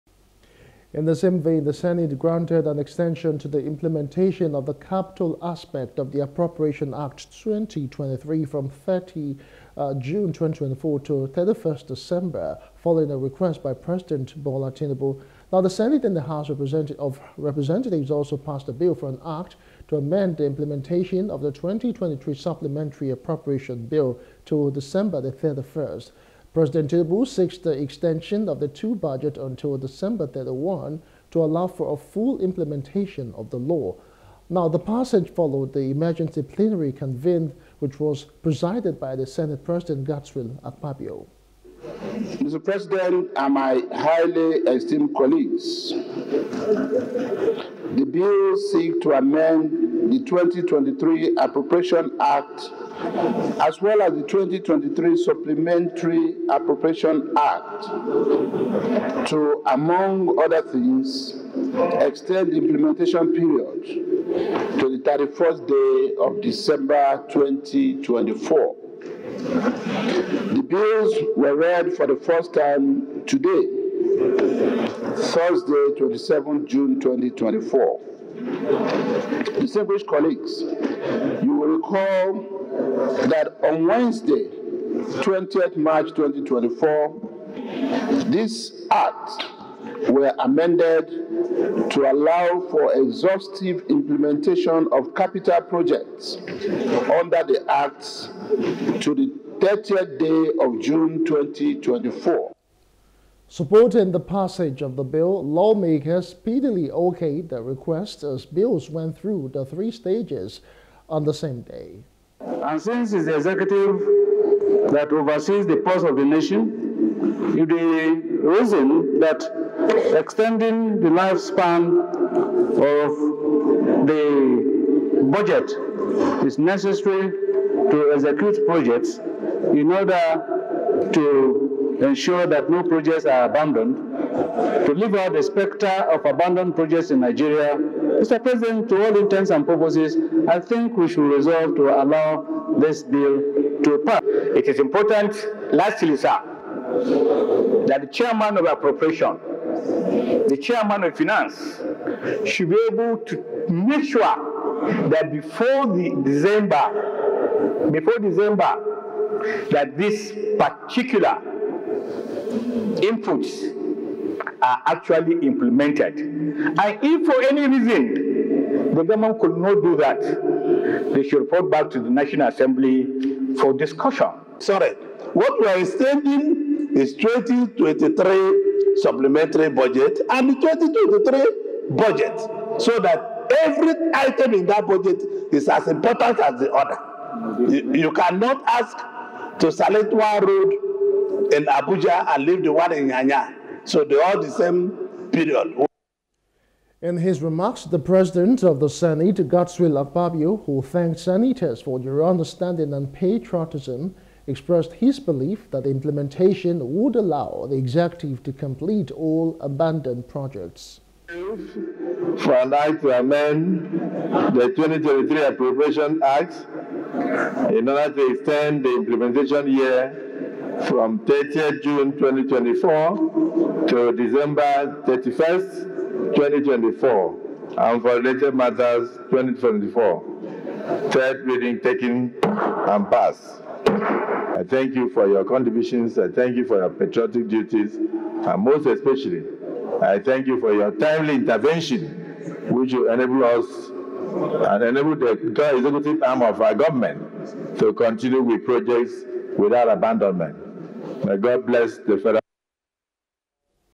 Budget Extension: Nass Passes 2023 Supplementary Appropriations Act Till 31st December
The National Assembly has granted an extension to the implementation of the Capital aspect of the Appropriation Act, 2023 from 30 June 2024 to 31 December 2024 following a request by President Bola Tinubu.